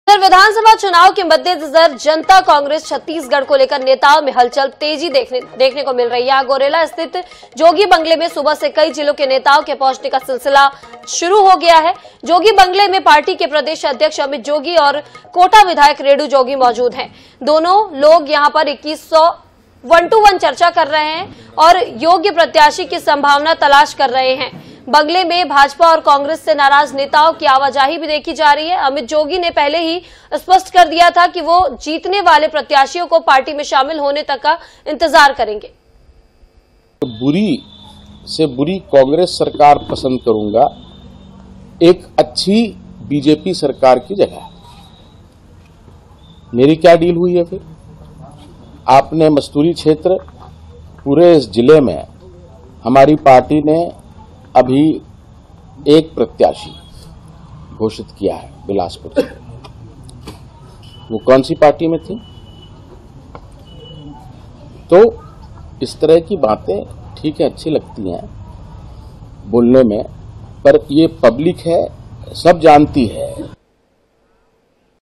Jogi बंगले में बढ़ी राजनीतिक हलचल | BJP-Congress के बागियों से संपर्क | CG Election 2023
INH, Jogi बंगले में बढ़ी राजनीतिक हलचल | BJP-Congress के बागियों से संपर्क | CG Election 2023

Incoming Search Terms: